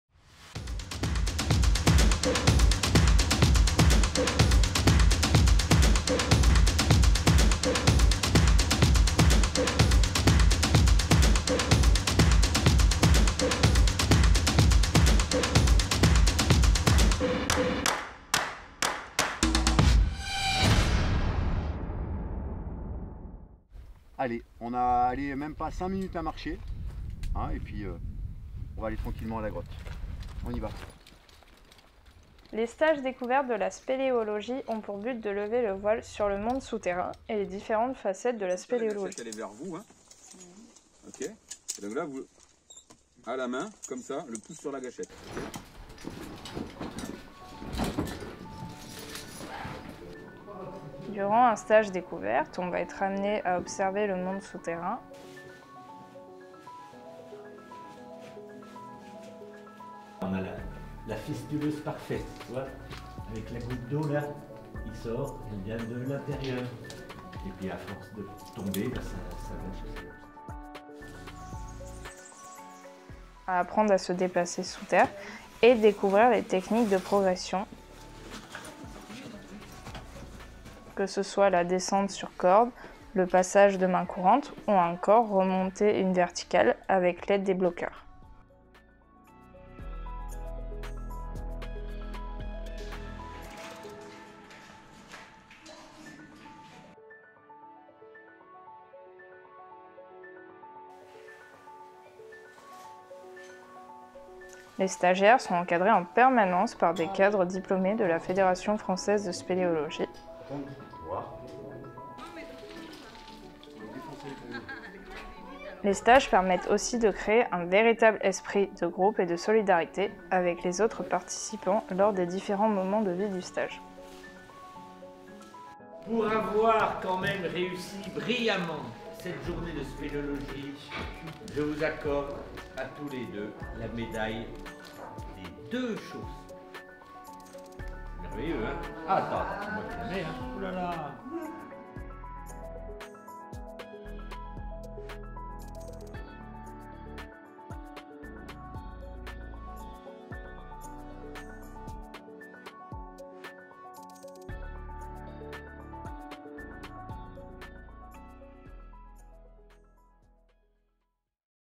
Stage découverte CSR BFC
Une semaine en stage de spéléologie avec la Fédération française de spéléologie, l’École française de spéléologie et le Comité spéléologique régional de Bourgogne-Franche-Comté.
Merci à Grégoire Limagne pour cette réalisation.